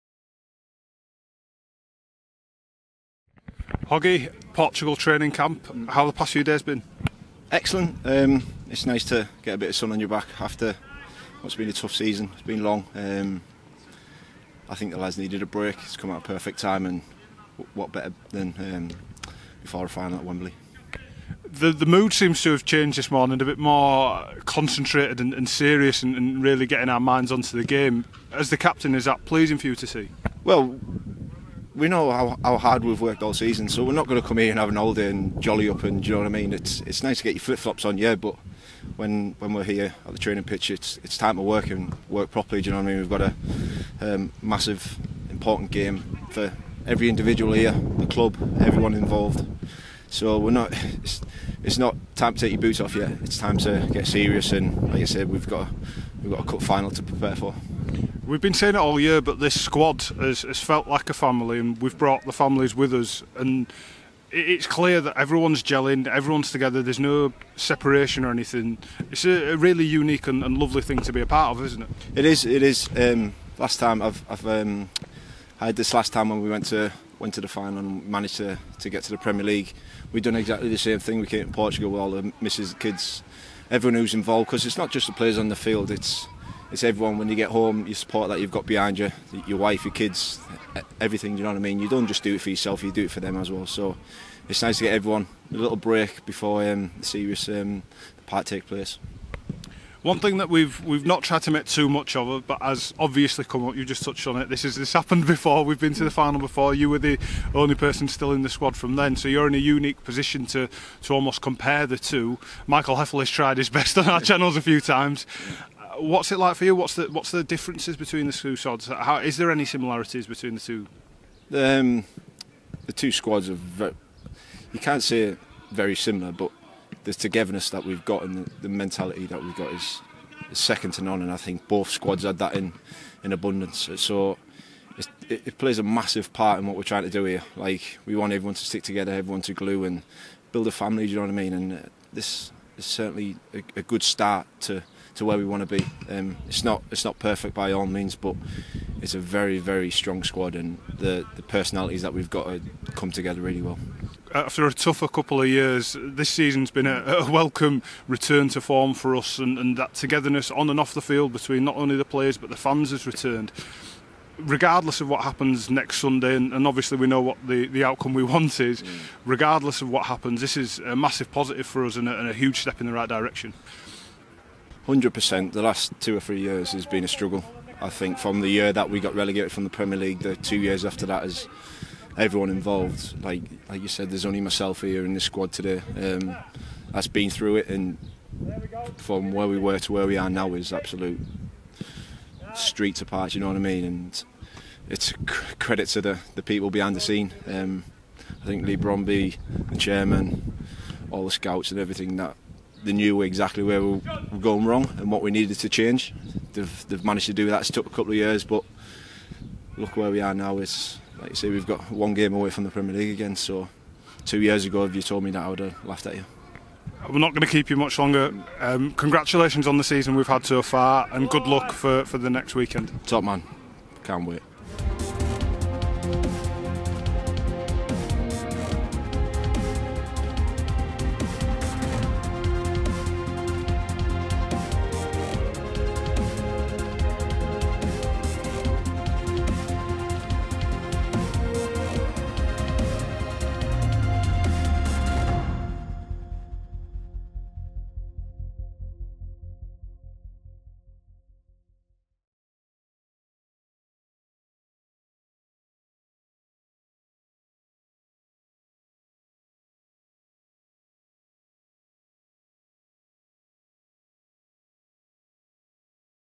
👊 THE GENERAL | PLAYER INTERVIEW | Jonathan Hogg on Portugal Training Camp & Wembley preparations
Jonathan Hogg spoke with HTTV from the training pitch at the Portugal Training Camp.